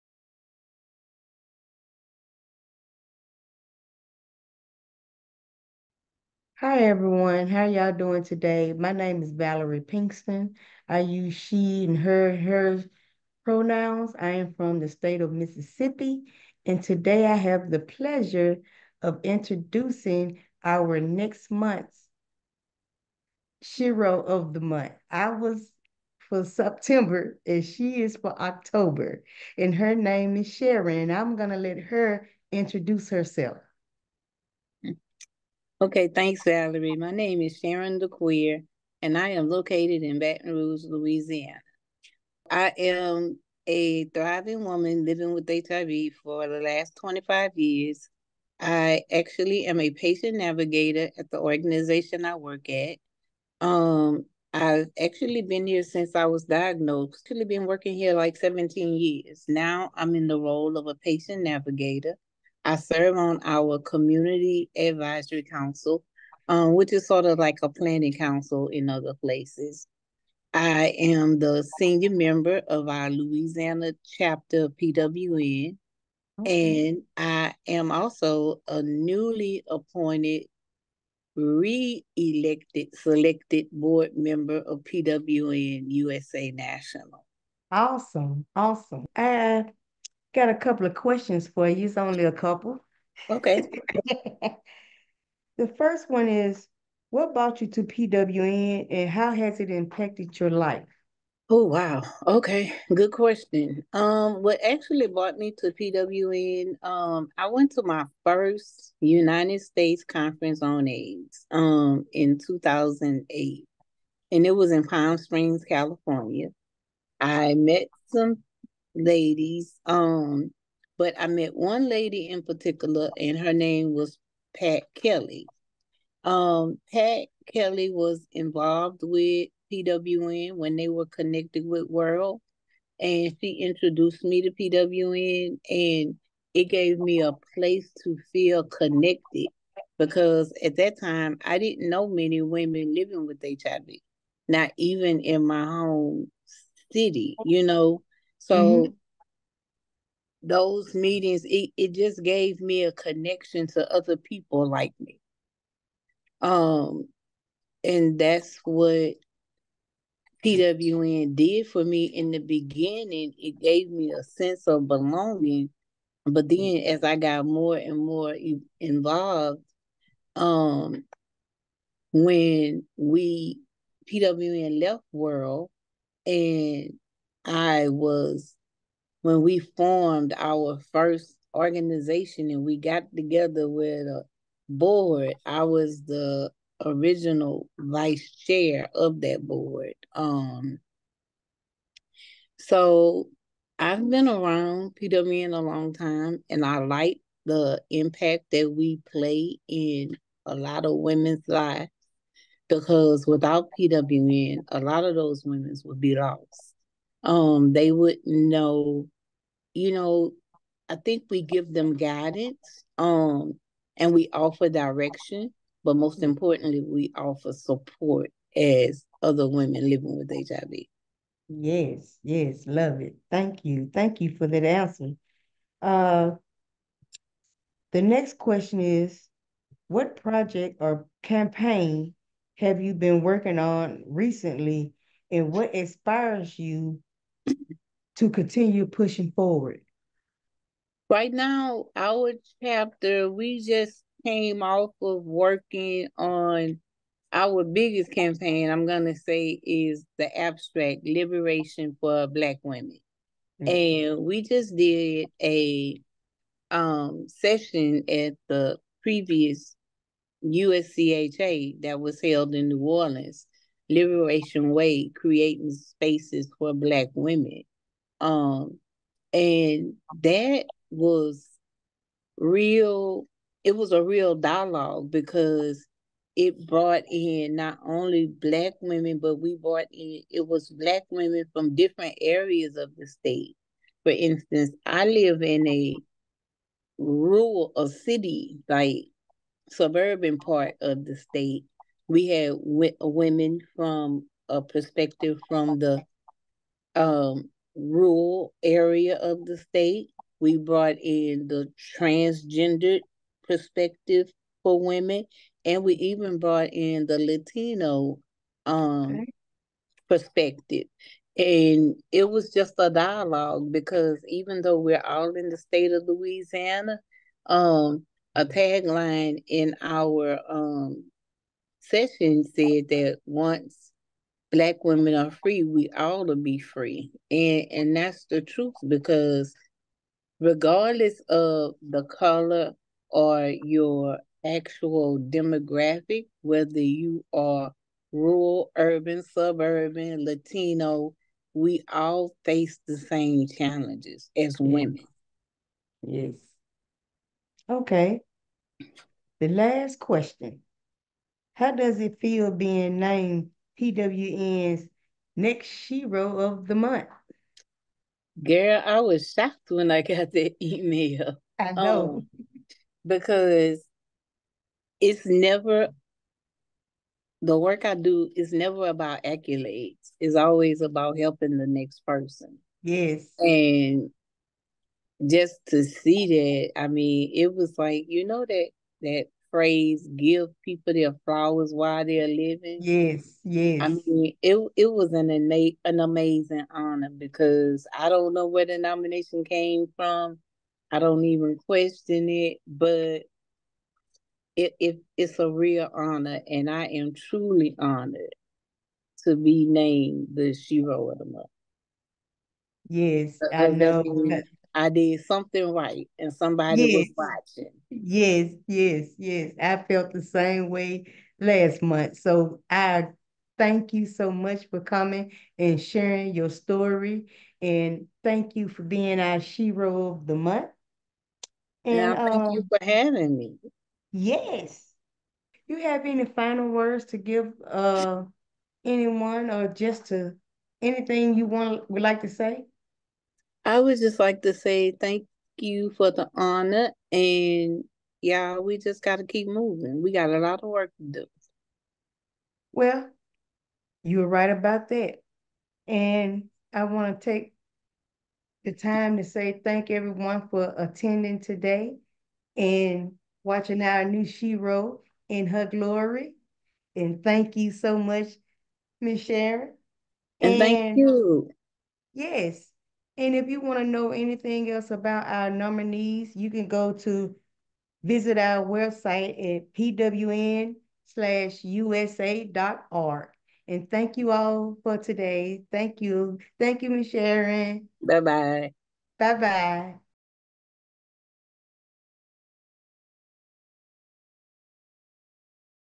Sharon DeCuir, PWN Shero of the Month - October 2024
Congratulations to Sharon DeCuir of Louisiana, PWN's Shero of the Month! Thank you for sharing about your beginnings with PWN and your powerful work with the Louisiana chapter at this year's USCHA. And a huge thank you to Valerie Pinkston of Mississippi, last month's Shero, who conducted this interview.